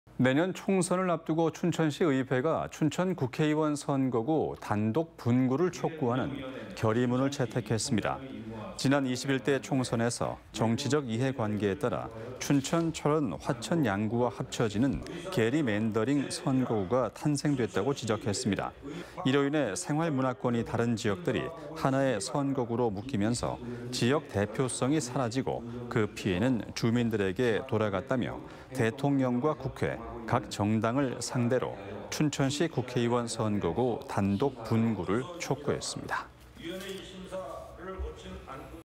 춘천시의회, 춘천 국회의원 선거구 단독 분구 촉구
내년 총선을 앞두고
춘천시의회가 춘천 국회의원 선거구
단독 분구를 촉구하는 결의문을 채택했습니다.

지난 21대 총선에서 정치적 이해 관계에 따라
춘천, 철원, 화천, 양구와 합쳐지는
게리멘더링 선거구가 탄생됐다고 지적했습니다.

이로 인해 생활문화권이 다른
지역들이 하나의 선거구로 묶이면서
지역 대표성이 사라지고
그 피해는 주민들에게 돌아갔다며,
대통령과 국회, 각 정당을 상대로
춘천시 국회의원 선거구
단독 분구를 촉구했습니다.